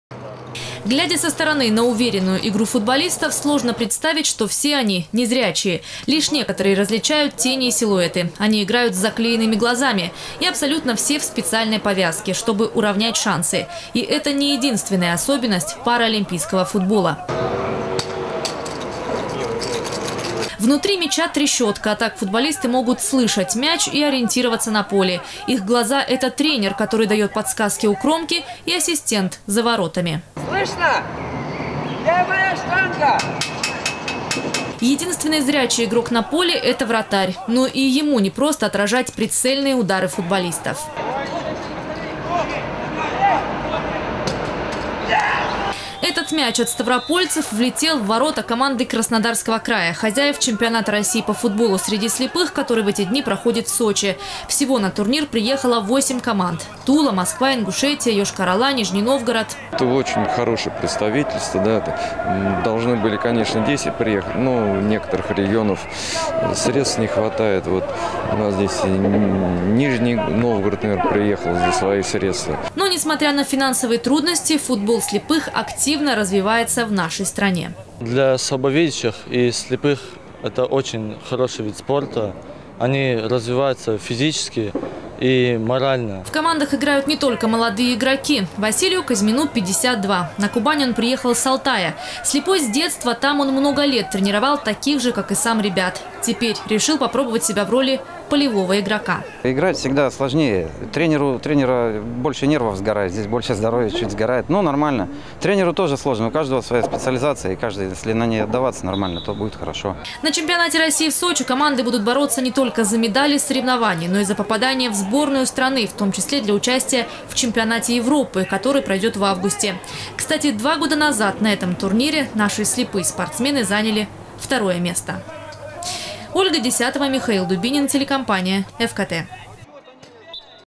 В Сочи проходит чемпионат России по футболу среди незрячих спортсменов. Новости Сочи Эфкате
Сегодня — второй игровой день. В Сочи на главный турнир страны прибыли 8 команд из разных регионов нашей страны.
Сочи. Новости.
Телеканал Эфкате.